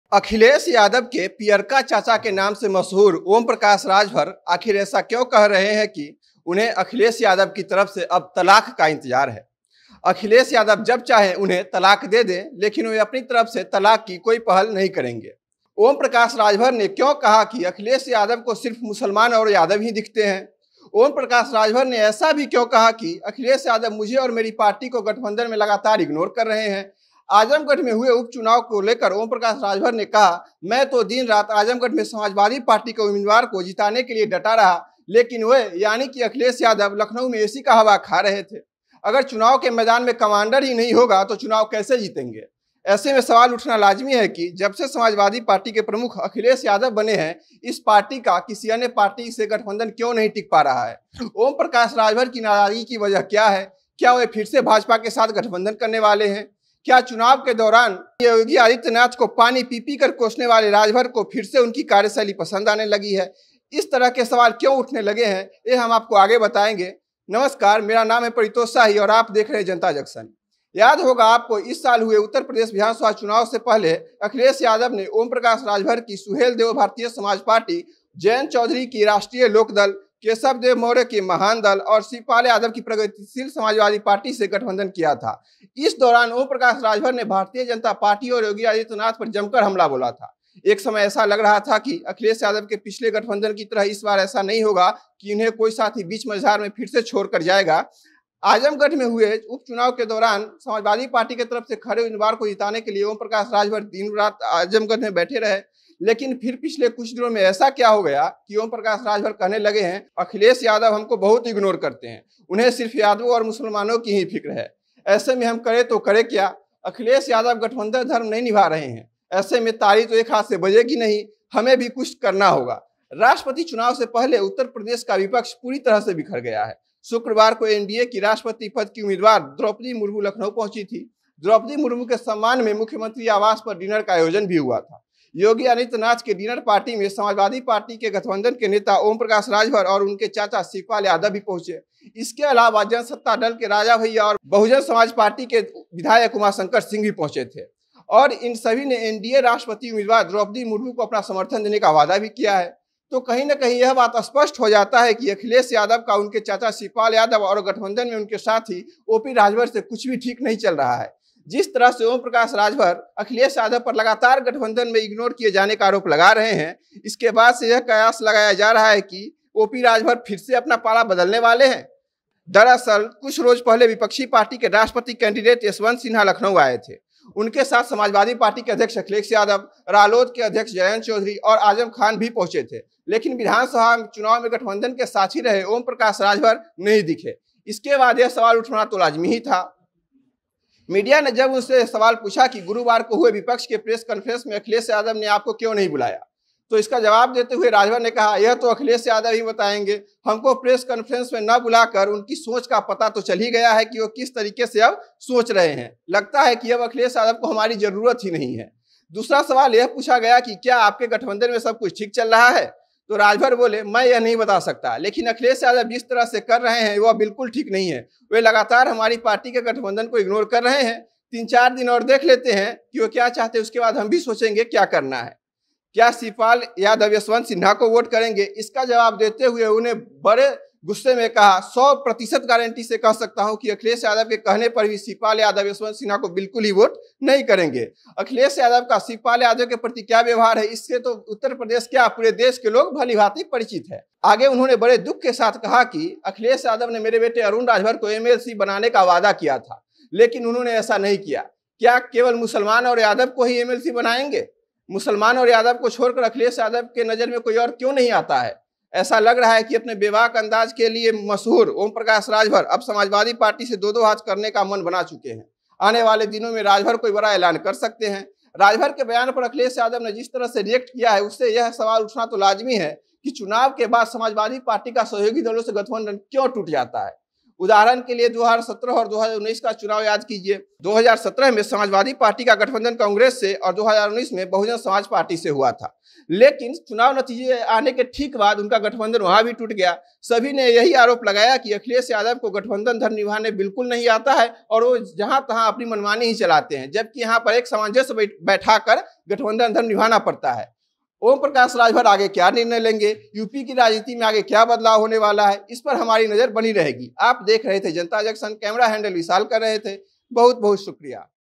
OP Rajbhar ने दिया Akhilesh Yadav को अंतिम अल्टीमेटम! बोले इतने दिन में तोड़ देंगे गठबंधन!
अखिलेश यादव द्वारा विपक्ष की मीटिंग में नहीं बुलाये जाने पर ओमप्रकाश राजभर बमक गए हैं।राजभर ने यहाँ तक कह दिया कि- अखिलेश को गठबंधन धर्म निभाने ही नहीं आता। जिसके कारण सब उनको छोड़ के जाते हैं।